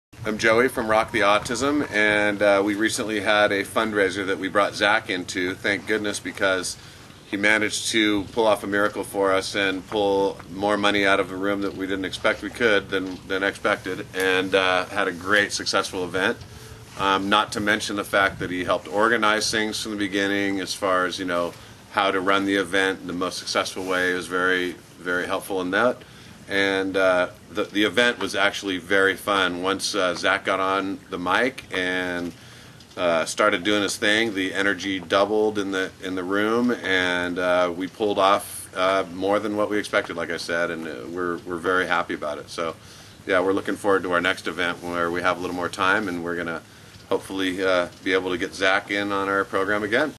Rock the Autism Testimonial of Zack Krone
Joey Santley The Managing Director of Rock the Autism a non profit educational group focusing on music education for families and children stricken with Autism speaks about his experience hiring and working with Auctioneer Zack Krone.  Zack Krone is a benefit auctioneer focusing on charity, non profit, health, educational and environmental causes